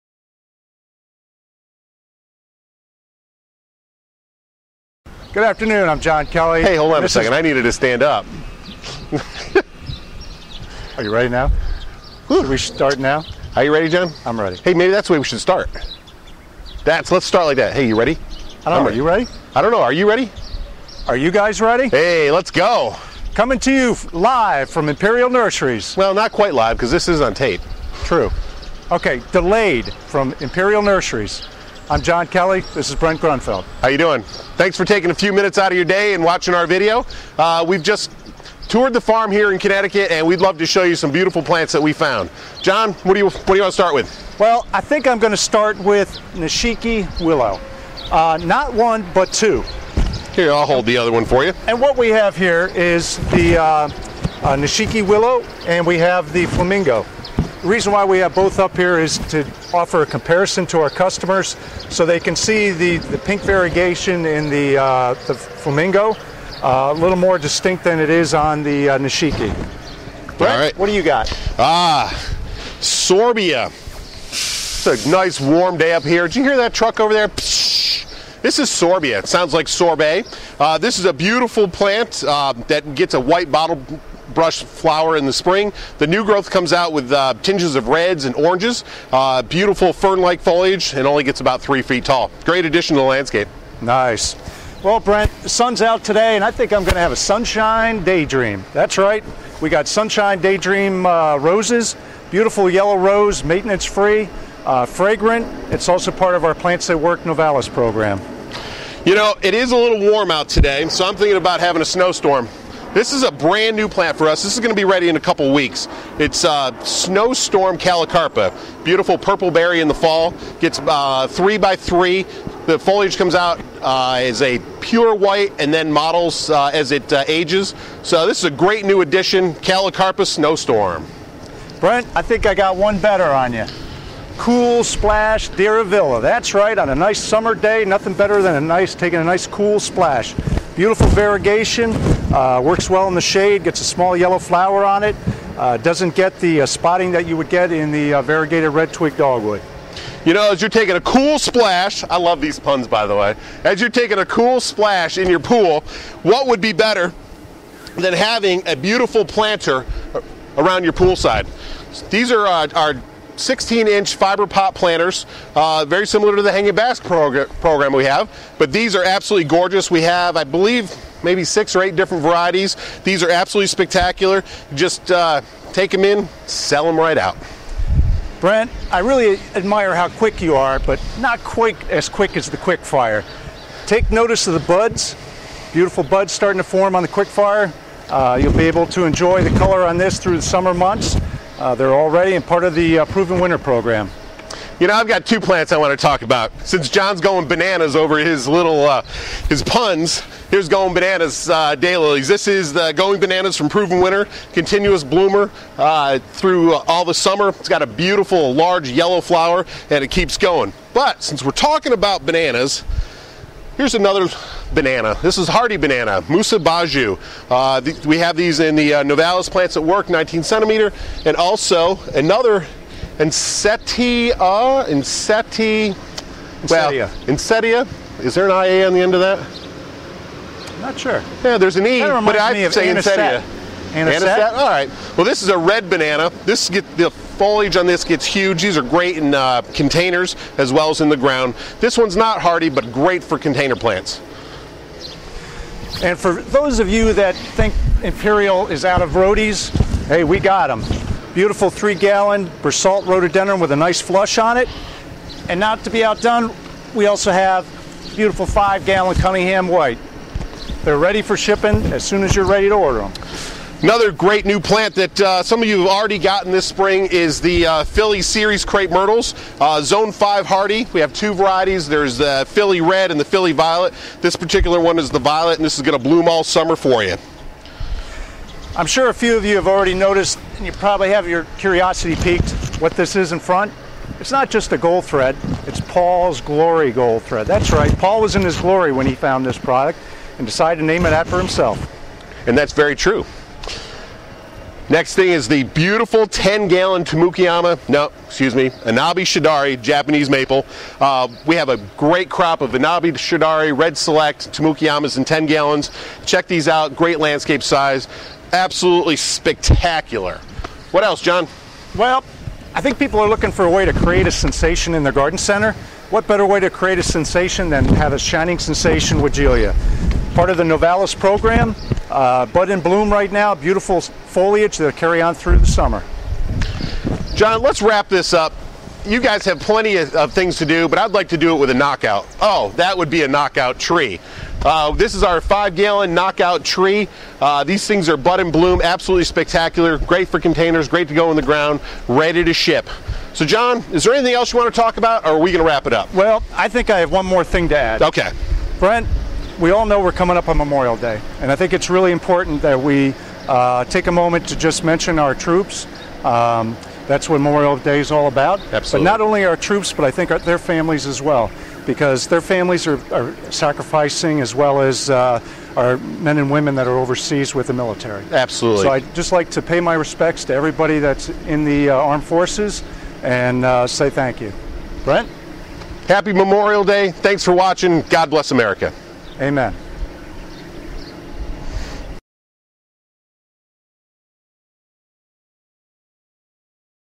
Farm Fresh Update and Happy Memorial Day from Brent & John at Imperial
After 2 days of touring Imperial's farm, Brent and John highlight some of the plants they discovered on the nursery!